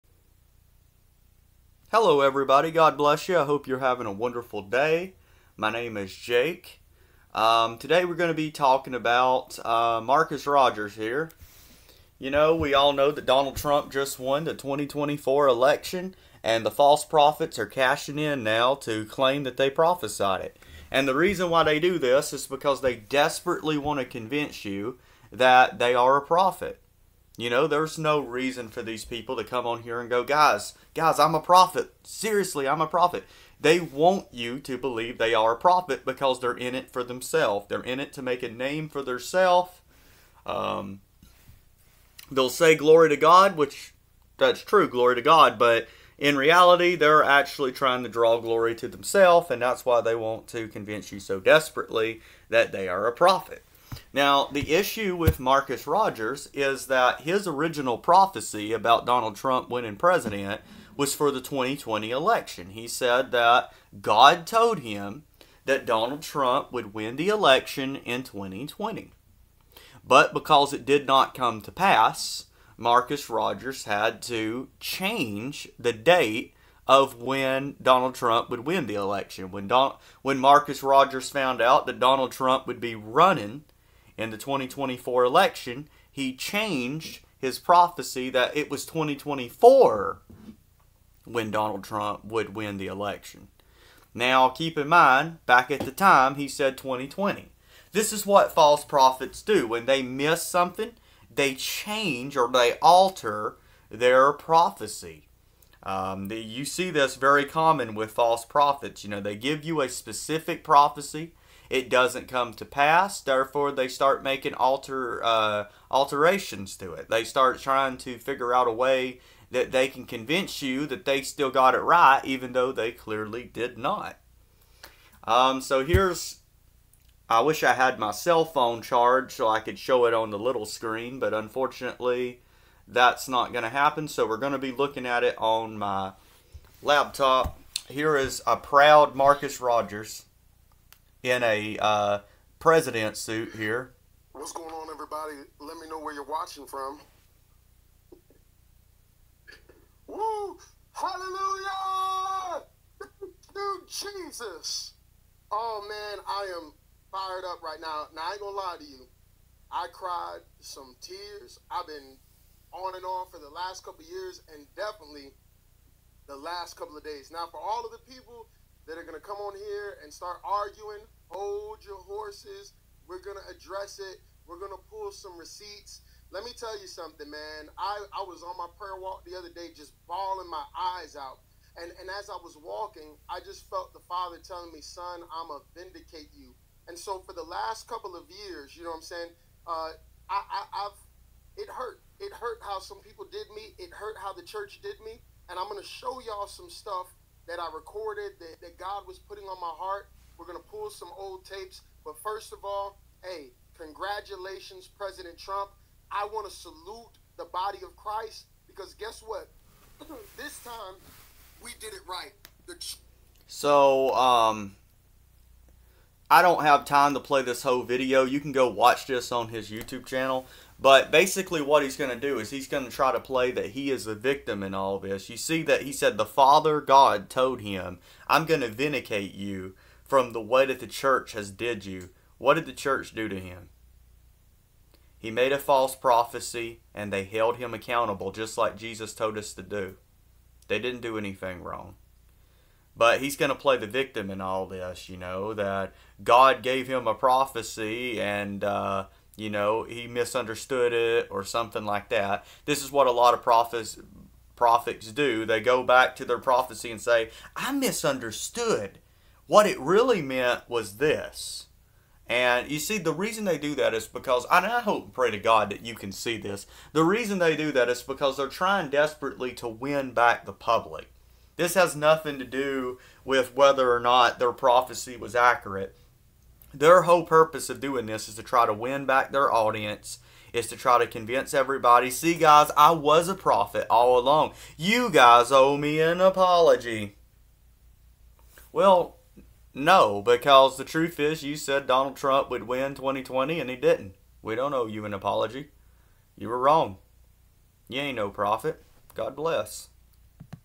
Beware of False trump prophets
Ezekiel 13:9
9 My hand will be against the prophets who see false visions and utter lying divinations. They will not belong to the council of my people or be listed in the records of Israel, nor will they enter the land of Israel. Then you will know that I am the Sovereign LORD.
Matthew 24:24
24 For false messiahs and false prophets will appear and perform great signs and wonders to deceive, if possible, even the elect.
Jeremiah 23:16
16 This is what the LORD Almighty says: “Do not listen to what the prophets are prophesying to you; they fill you with false hopes. They speak visions from their own minds, not from the mouth of the LORD.
Jeremiah 14:14
14 Then the LORD said to me, “The prophets are prophesying lies in my name. I have not sent them or appointed them or spoken to them. They are prophesying to you false visions, divinations, idolatries and the delusions of their own minds.

REMEMBER EVERYONE, I bear no hatred towards anyone we discuss or warn people about. It is strictly to help warn people about those who wish to deceive and take advantage of the body of Christ. My Prayer for Marcus is that he drops the PROPHET act, and focuses on following Jesus and leading people to Salvation. Let noone think for a second, that I am wishing any harm upon these people.

May Jesus Bless U